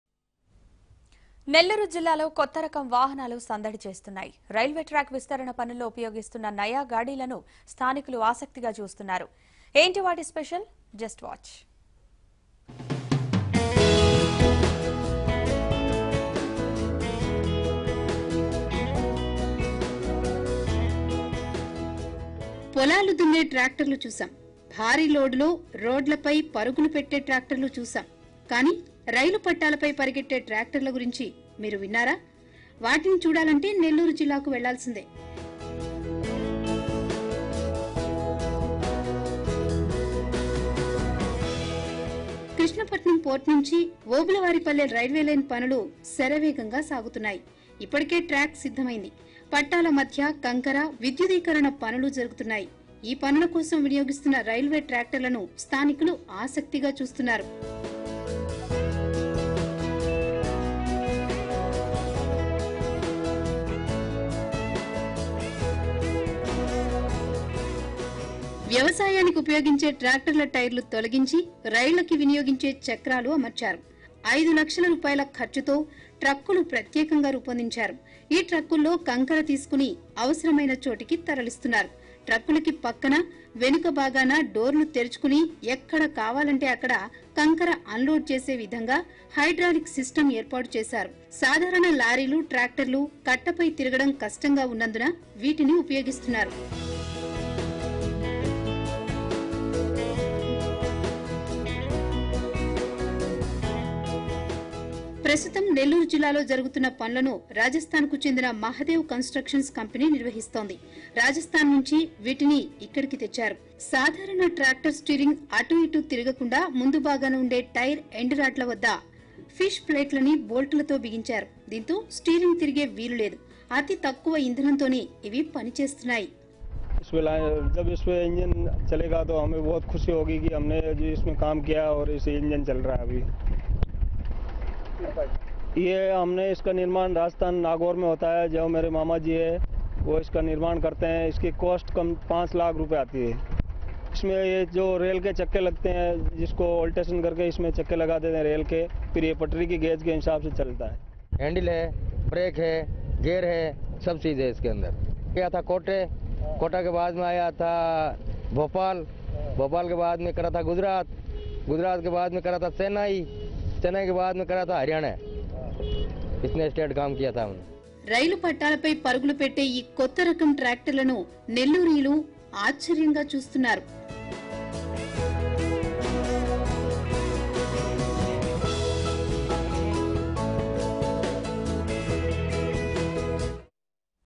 Tractors Fitted with Train Wheels Being Used in Railway Line Works | Nellore District | ABN Telugu
A new idea has bee n implemented by the Krishnapatnam port people in Nellore as the tractor is being fitted with train wheels and are being used to commute on tracks for shifting the raw material like small stones and also for the transportation of coal and many more.